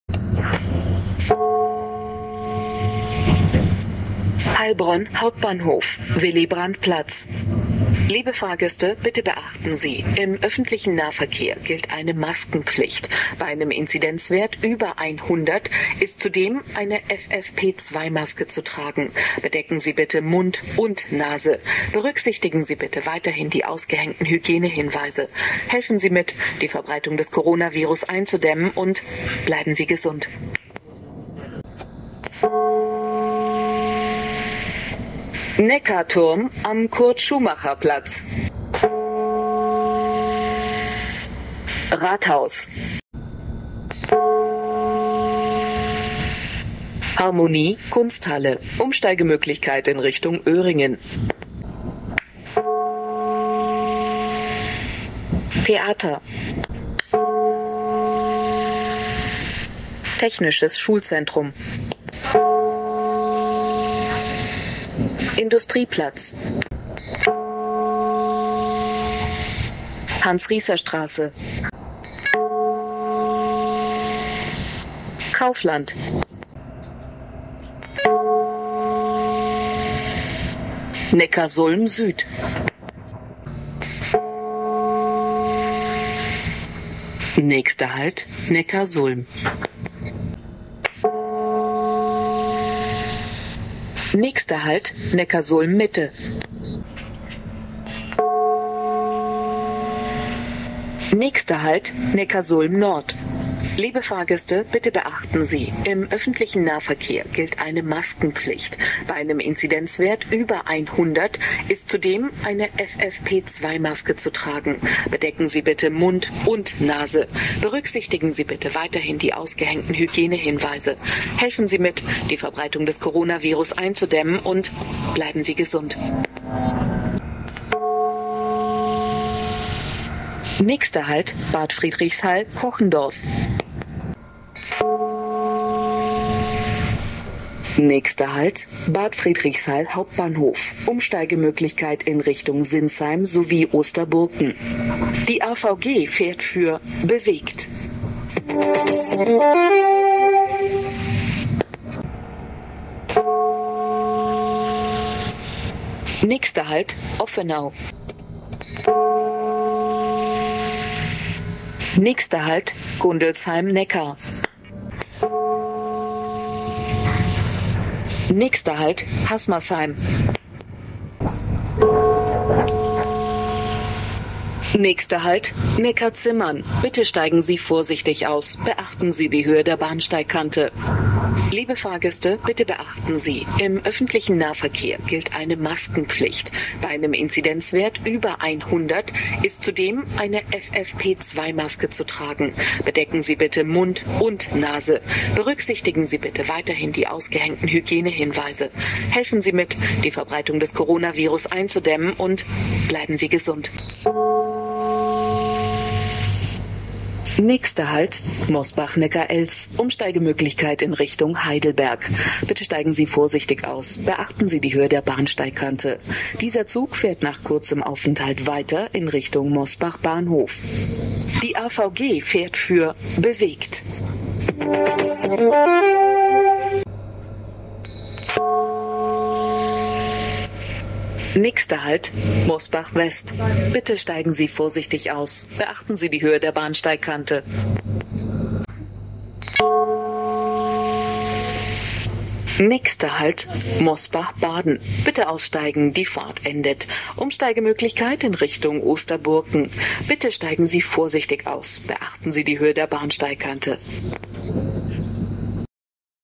S41 Ansagen der HNV/KVV. Gesprochen von Anneta Politi
Herzlich willkommen zu meinem neuen Video mit allen Ansagen der Linie S41. Gesprochen von Anneta Politi